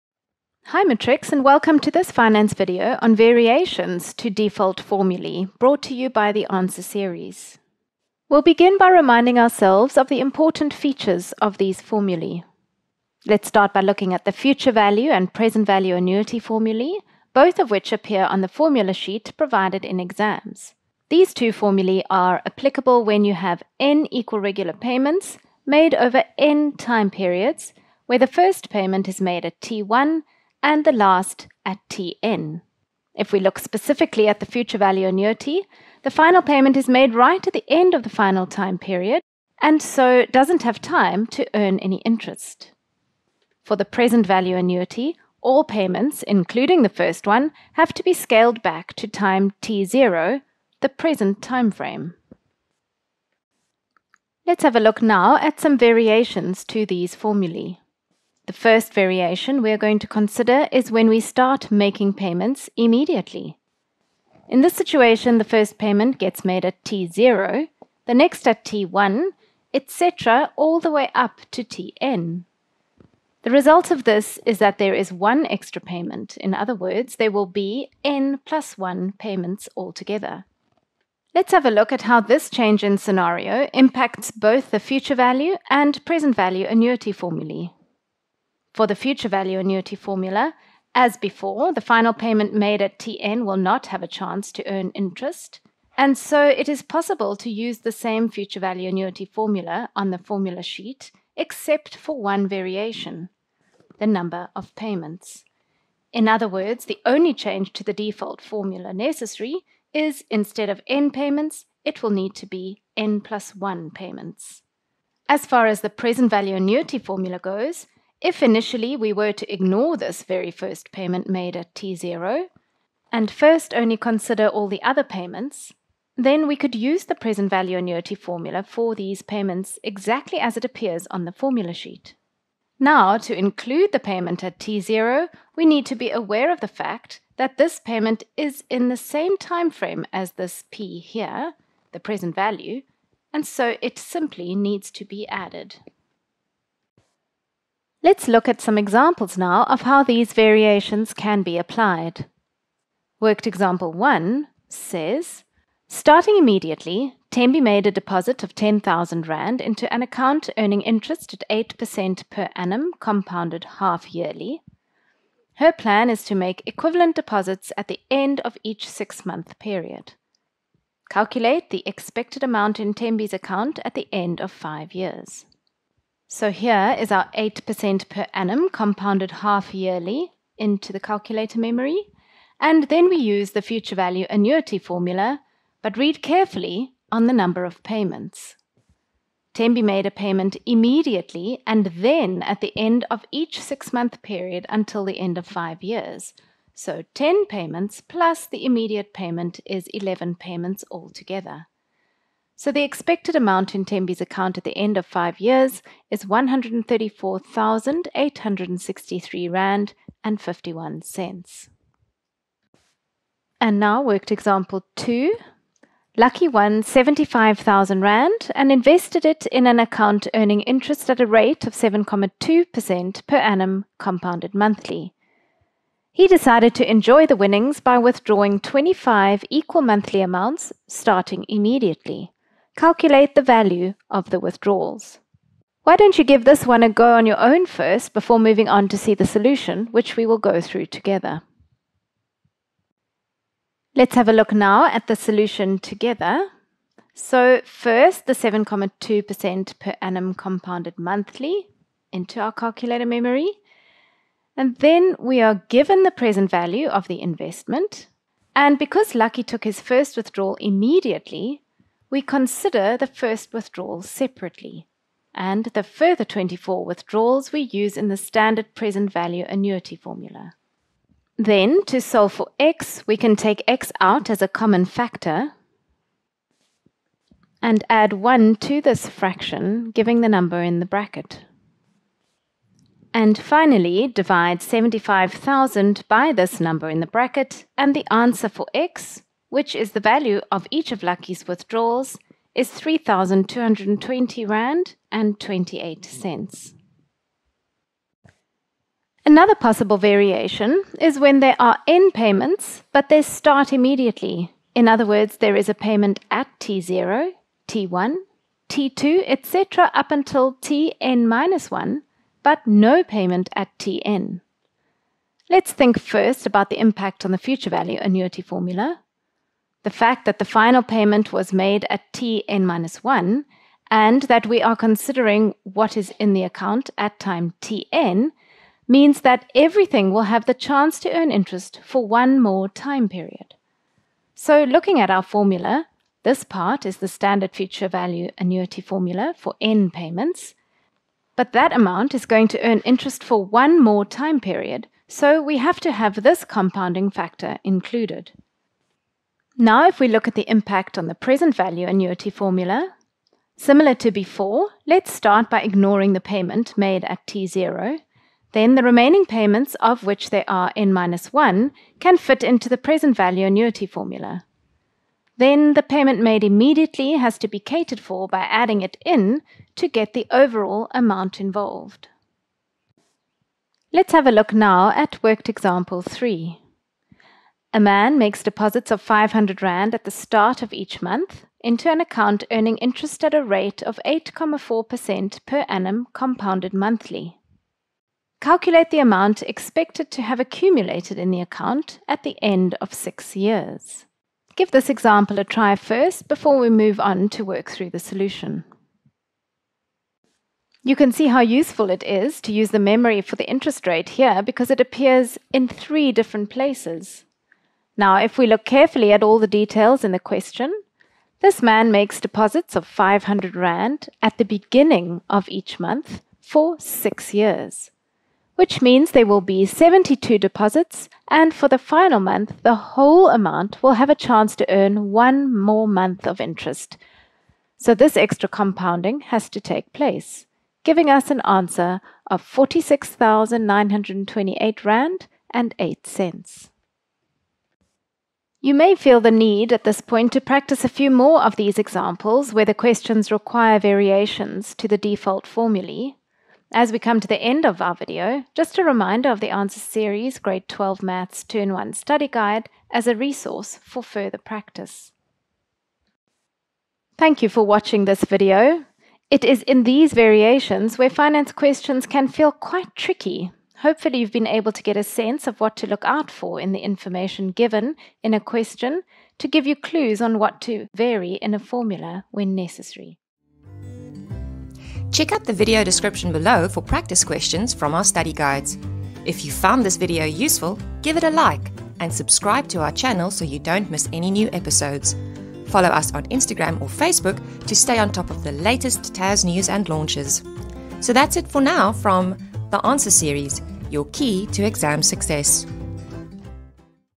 GRADE 12 MATHS FINANCE – Variations on Default Formulae #13
This video clarifies how adjustments need to be made to the default formulae, depending on when payments are made.

Practice Questions for this video
Gr 12 Mathematics 2-in-1
• Page 18: Questions 11 &12
• Page 150: Paper A1 – Question 7.1